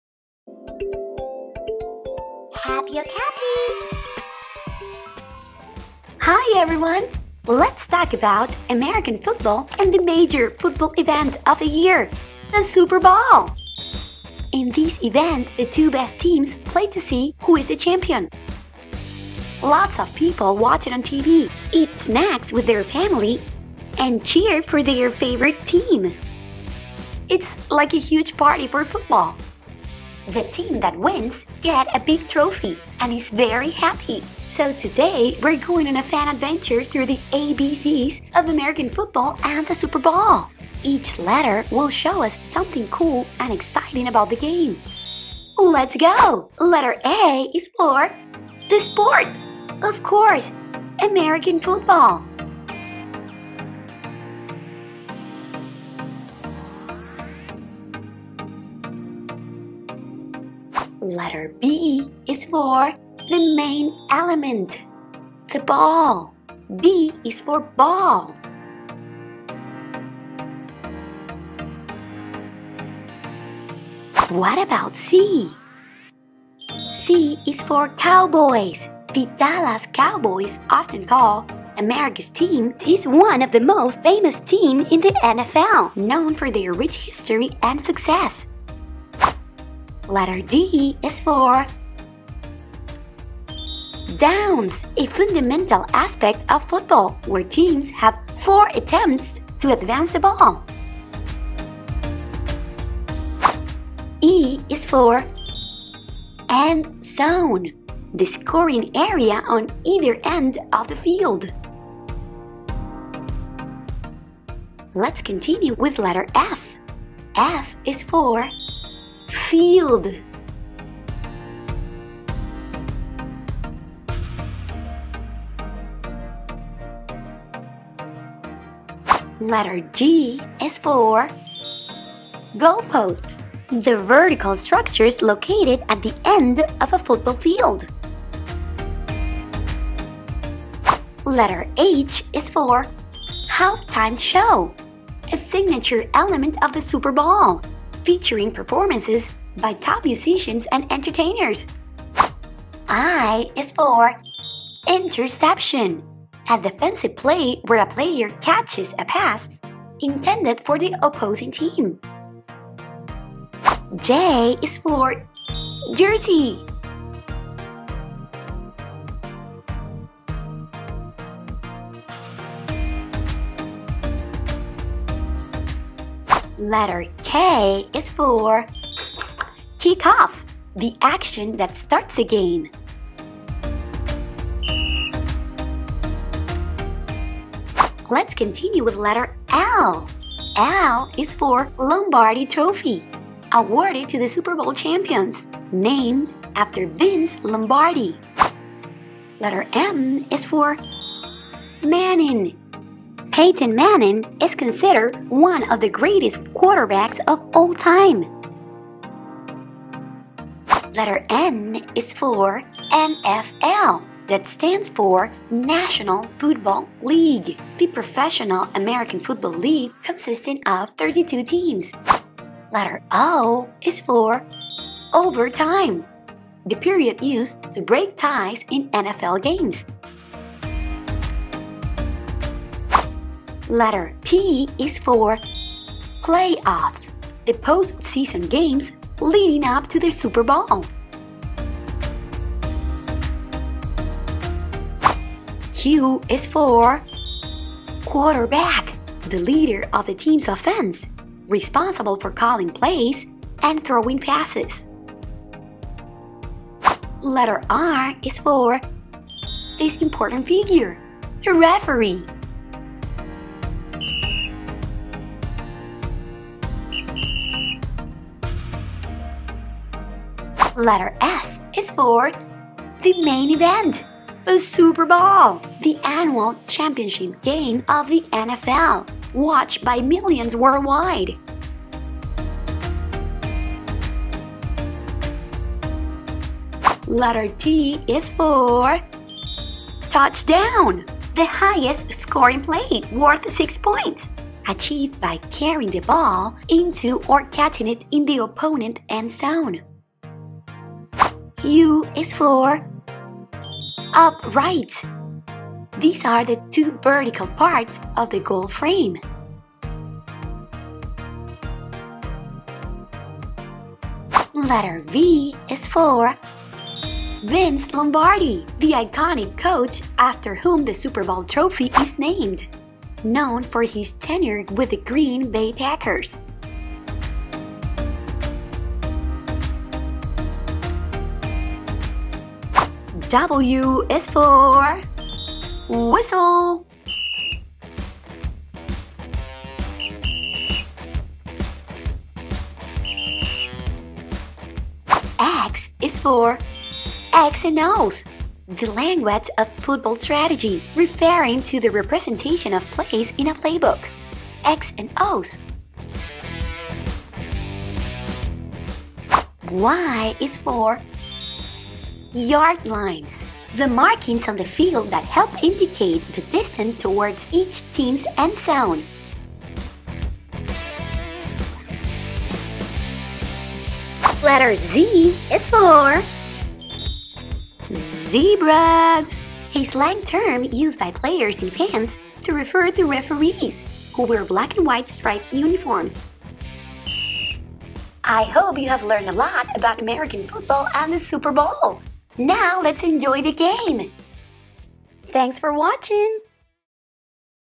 Super BOWL ABC 🏈🎉 - Learn the Alphabet with American Football
Hey, little friends! Today, we're going on a fun adventure through the ABCs of American Football and the Super Bowl! 🏈🎉

Each letter will show us something cool and exciting about the game, from awesome players, main terms, to the big, shiny trophy.
So, grab your favorite snack, put on your team colors, and let's jump into this playful journey from A all the way to Z.

Are you ready to learn and have fun with the alphabet of football? Let's go!

A is for. American Football.
C is for. Cowbows, America's Team, the Dallas Cowboys

Wanna keep learning?